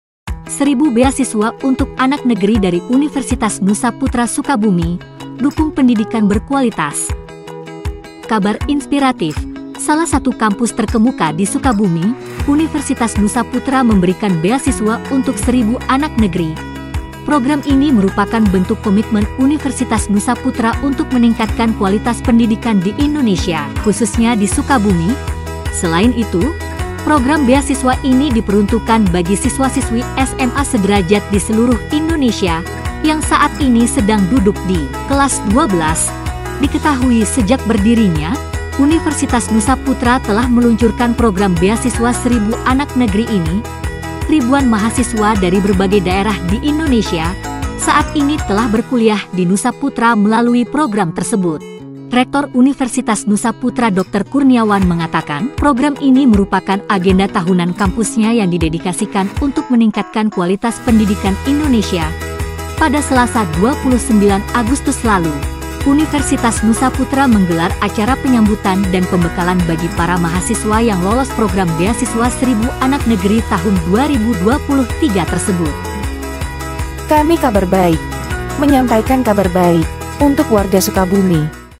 1.000 BEASISWA UNTUK ANAK NEGERI DARI UNIVERSITAS NUSA PUTRA SUKABUMI, DUKUNG PENDIDIKAN BERKUALITAS
Kabar inspiratif, salah satu kampus terkemuka di Sukabumi, Universitas Nusa Putra memberikan beasiswa untuk 1.000 anak negeri.

Program ini merupakan bentuk komitmen Universitas Nusa Putra untuk meningkatkan kualitas Pendidikan Indonesia, khususnya di Sukabumi.

Selain itu, program beasiswa ini diperuntukan bagi siswa-siswi SMA sederajat di seluruh Indonesia yang saat ini sedang duduk di kelas XII.

Diketahui sejak berdirinya, Universitas Nusa Putra telah meluncurkan program Beasiswa 1.000 Anak Negeri ini.

Ribuan mahasiswa dari berbagai daerah di Indonesia saat ini telah berkuliah di Nusa Putra melalui program tersebut.

Rektor Universitas Nusa Putra Dr. Kurniawan mengatakan, program ini merupakan agenda tahunan kampusnya yang didedikasikan untuk meningkatkan kualitas Pendidikan Indonesia.

Pada Selasa 29 Agustus lalu, Universitas Nusa Putra menggelar acara penyambutan dan pembekalan bagi para Mahasiswa yang lolos Program Beasiswa 1000 Anak Negeri tahun 2023 tersebut.